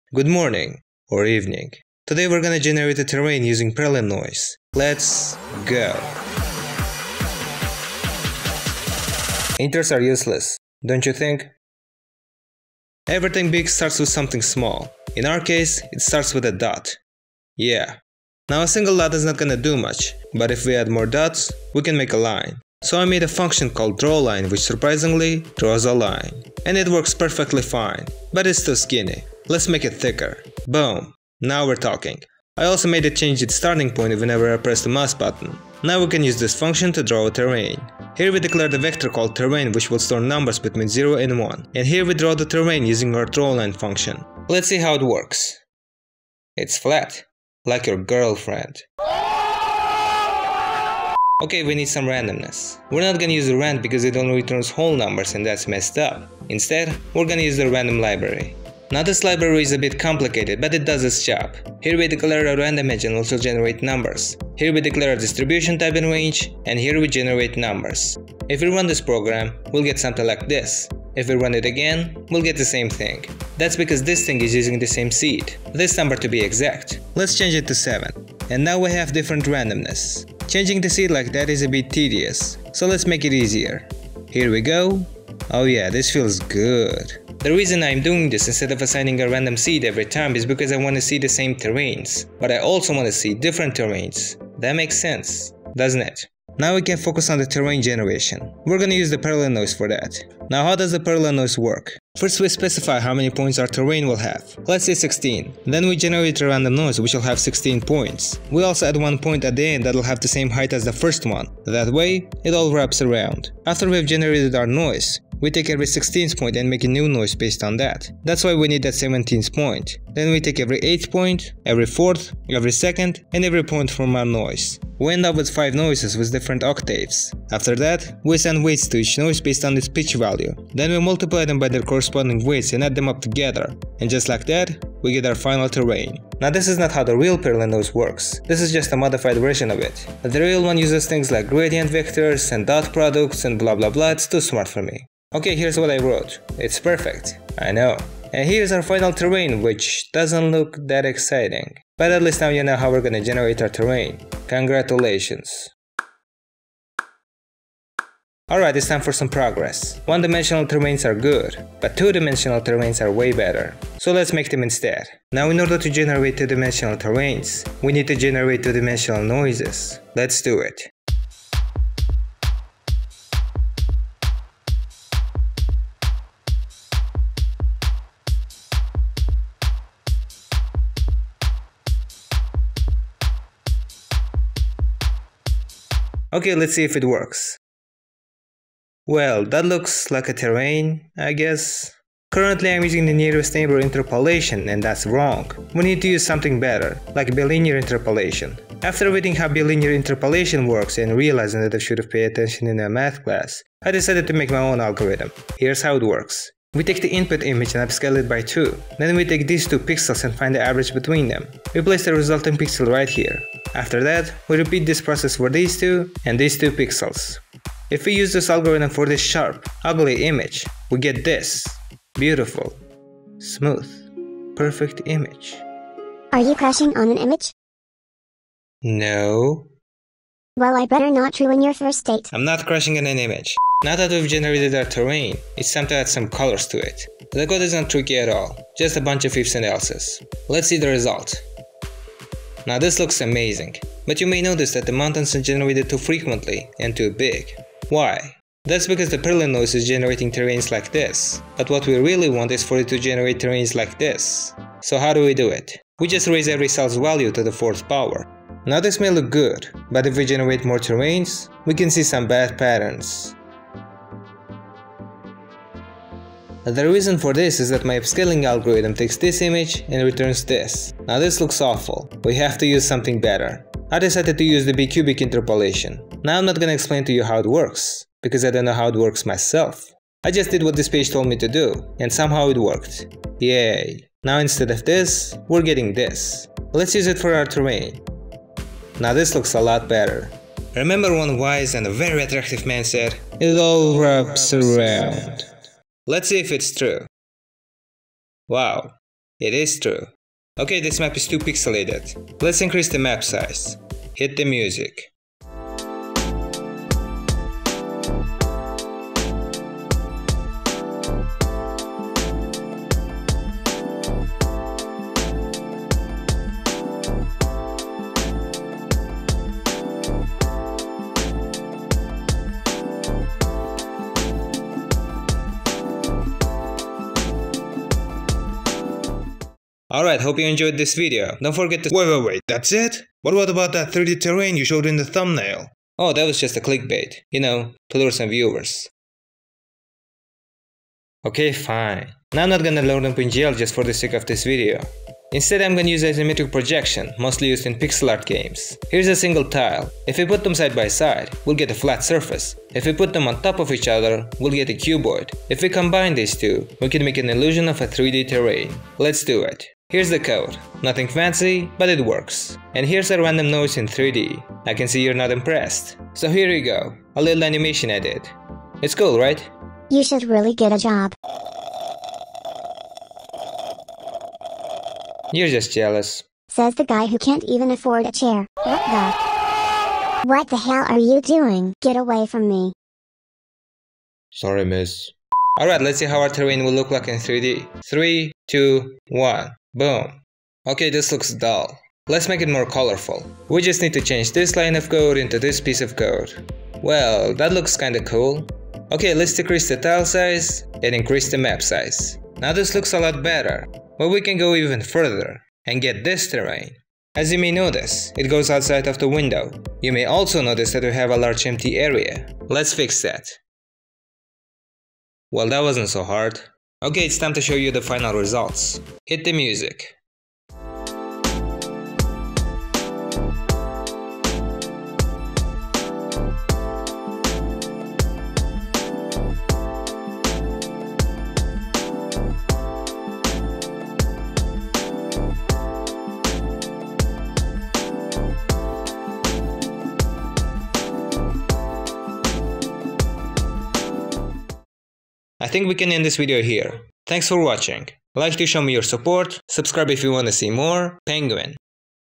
Terrain Generation Using Perlin Noise
Made a terrain generator using Perlin noise.